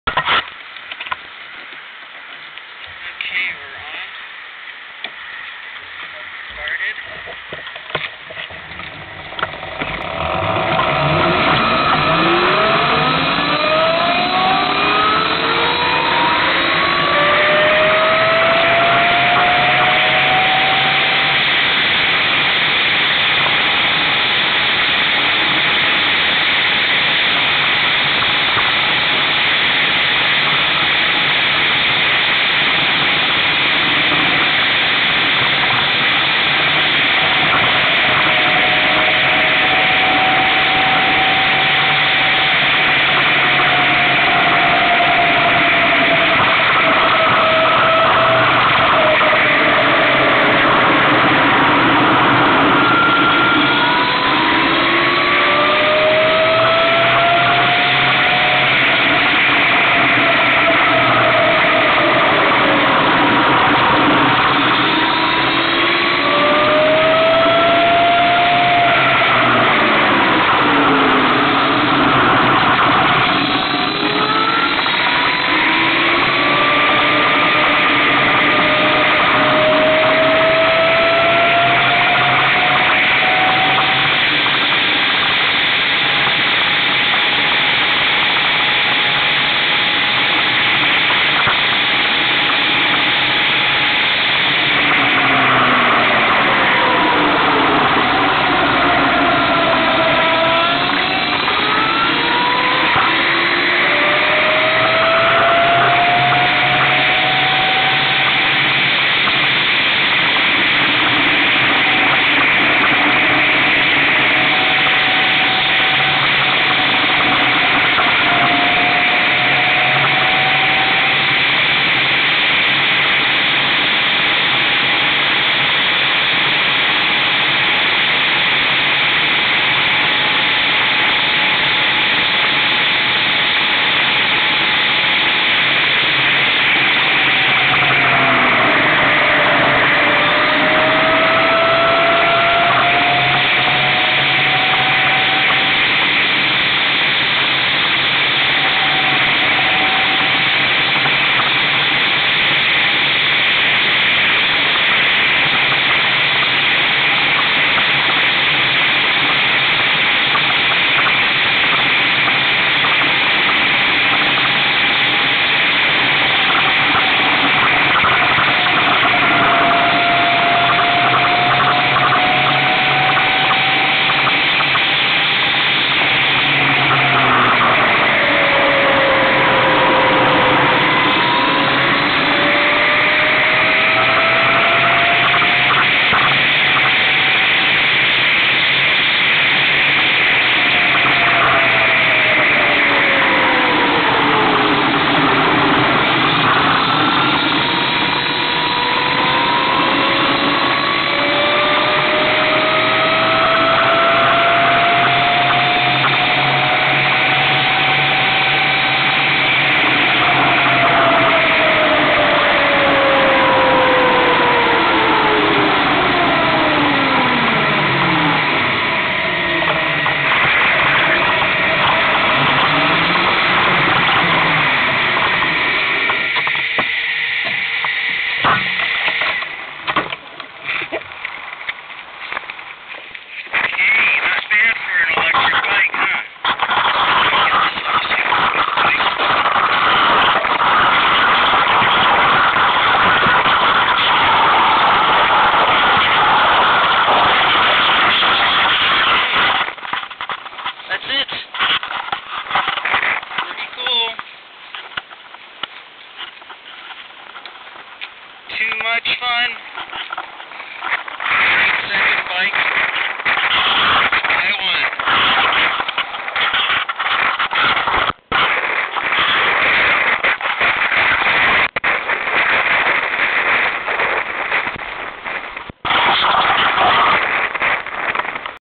Lloyd's 2009 Brammo Enertia
A quick spin on my electric motorcycle.  I have 3 bikes at home and this one is the most fun for local riding.  It is a lot quieter than my Buell!